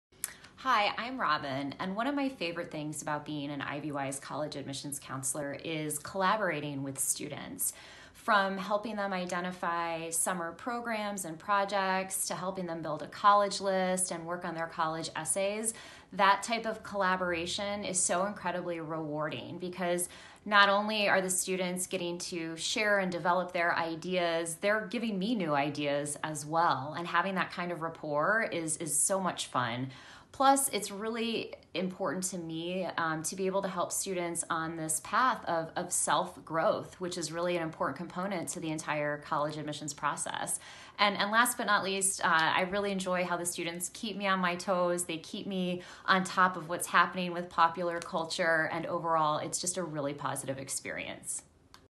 👋Meet Robin: IvyWise Premier College Admissions Counselor
Meet Robin, IvyWise Premier College Admissions Counselor and former Senior Assistant Director of Admission, Georgetown University.